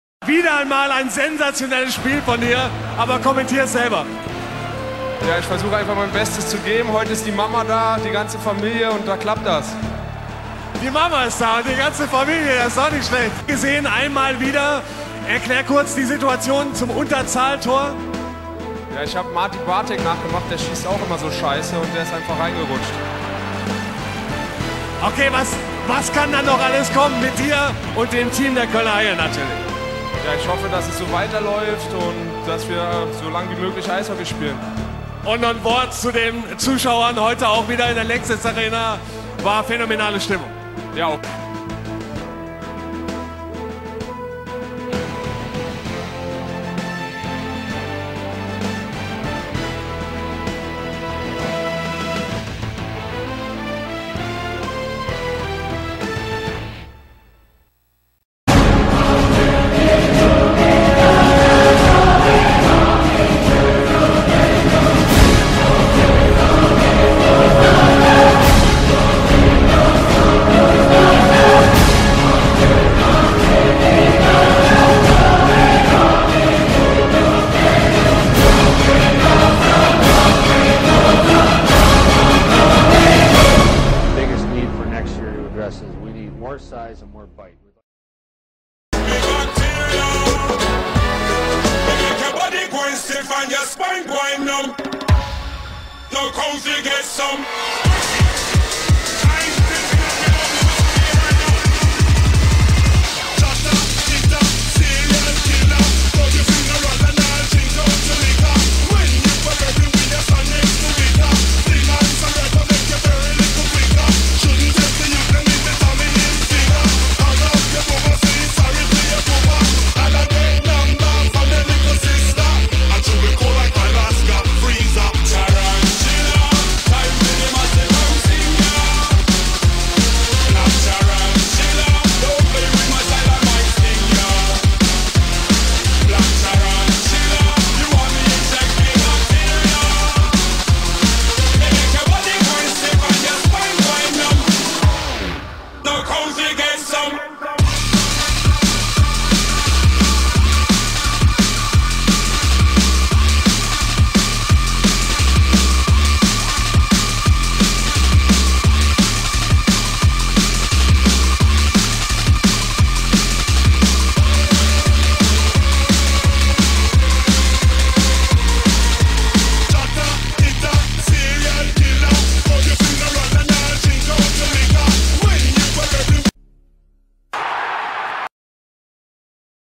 Marcel Müller welcome to NHL Tribute
Marcel Müller #9 from Kölner Haie is one of the new Player for the Toronto Maple Leafs! Season 10/11